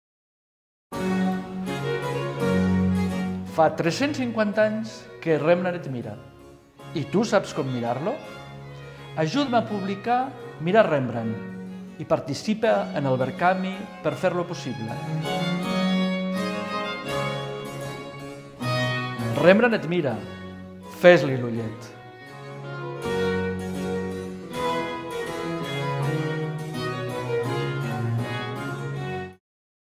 Mirar Rembrandt
Fa 350 anys que Rembrandt et mira. I tu saps com mirar-lo?
Ajuda l'Antoni Gelonch a publicar "Mirar Rembrandt" i participa en el Verkami per fer-lo possible.
Rembrandt et mira, fes-li l'ullet!